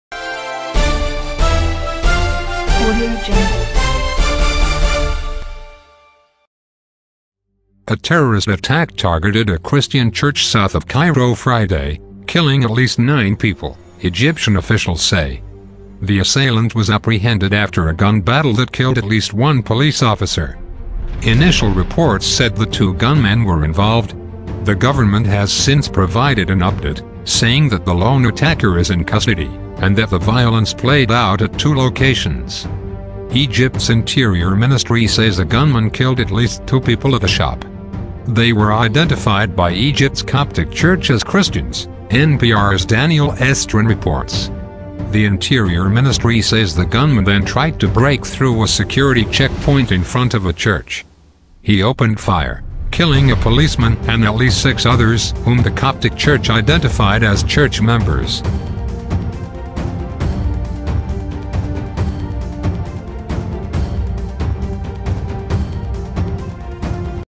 Egypt says 10 killed in attack outside Cairo church
A terrorist attack targeted a Christian church south of Cairo Friday, killing at least nine people, Egyptian officials say. The assailant was apprehended after a gun battle that killed at least one police officer.

Initial reports said that two gunmen were involved; the government has since provided an update, saying that the lone attacker is in custody, and that the violence played out at two locations.

"Egypt's interior ministry says a gunman killed at least two people at a shop. They were identified by Egypt's Coptic church as Christians," NPR's Daniel Estrin reports. "The interior ministry says the gunman then tried to break through a security checkpoint in front of a church. He opened fire, killing a policeman and at least six others whom the Coptic church identified as church members."

Despite the death toll, the state news agency MENA is calling the attack a foiled attempt; Egyptian media report that along with guns, the assailants were also carrying explosives, suggesting that the violence could have been far worse.

The violence centered on the Mari Mina Church, in the city of Helwan on the southern outskirts of Cairo, east of the Nile River.

The attack comes amid heightened security, as Egyptian Christians, who are roughly 10 percent of the country's population, prepare to celebrate the Orthodox Christmas, which is observed one week into the new year.

Earlier this week, Egypt's Ministry of Defense said that it's mobilizing part of the armed forces to help provide security for Christmas celebrations.

The U.S. Embassy in Cairo issued a tweet saying it "strongly condemns today's terrorist attack on a church."

The statement continued, "We offer condolences to the families of the victims and wishes for speedy recovery for those wounded. The U.S stands steadfastly with the people of Egypt in the face of such cowardly attacks."

   cannel name --